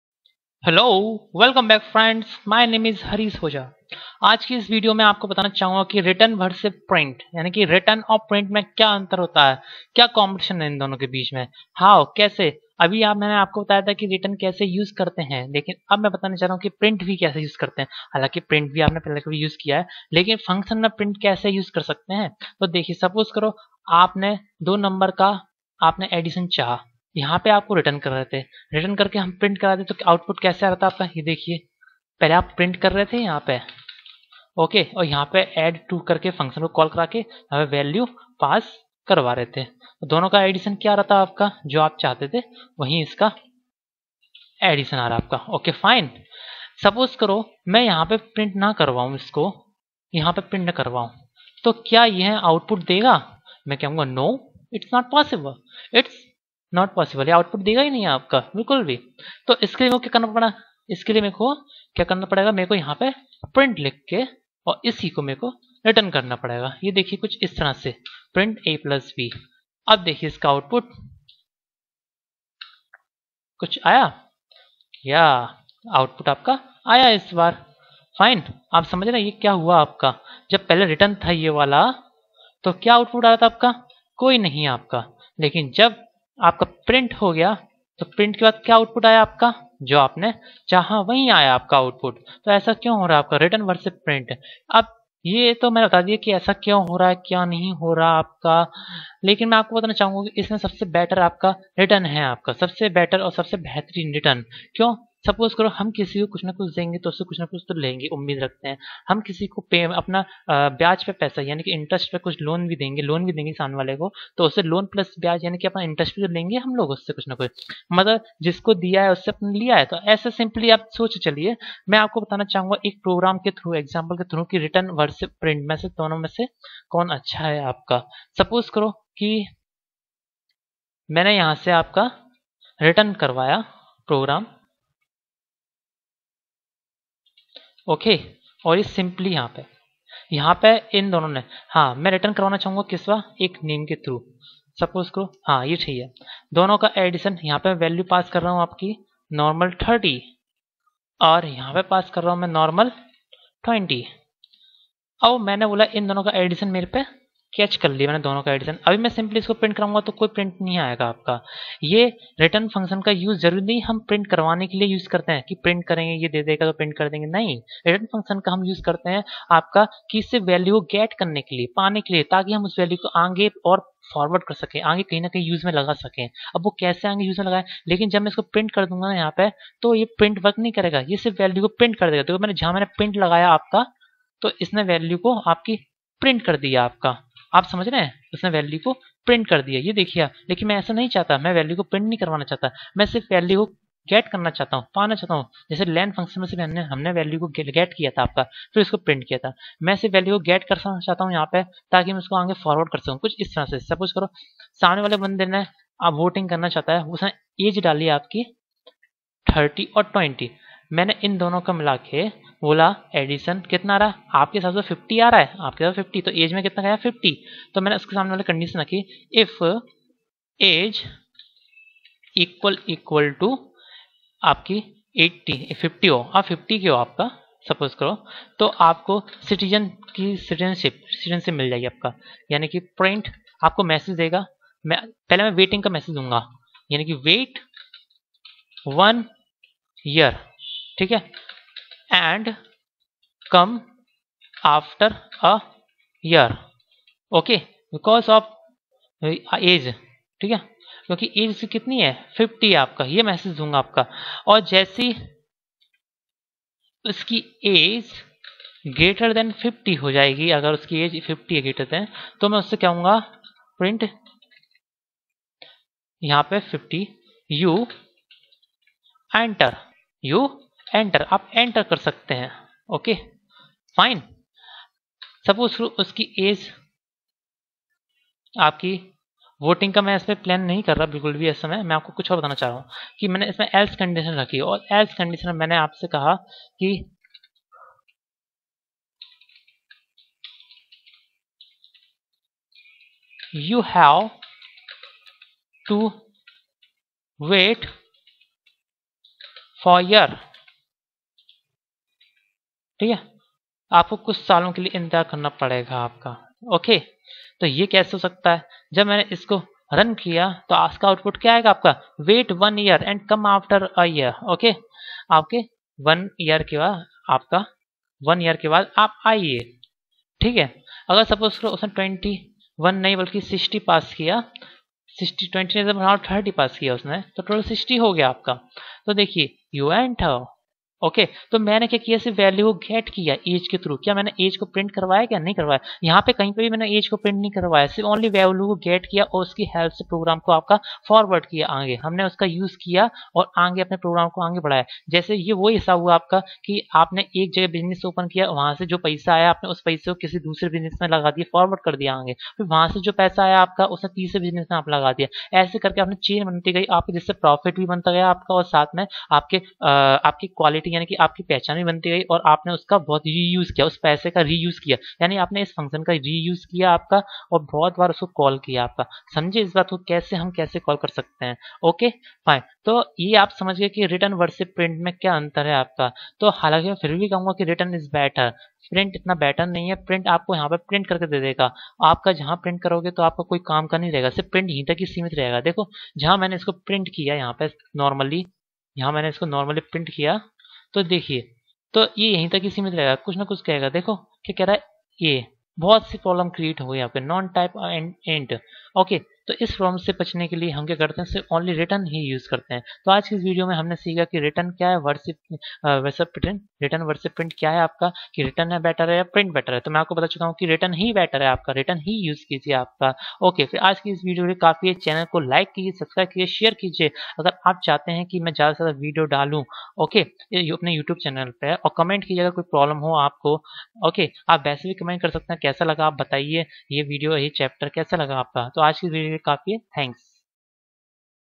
Return Vs Print in function of Python
This video is most important to understand which is better Return or Print...
To understand function concept ... follow below link ---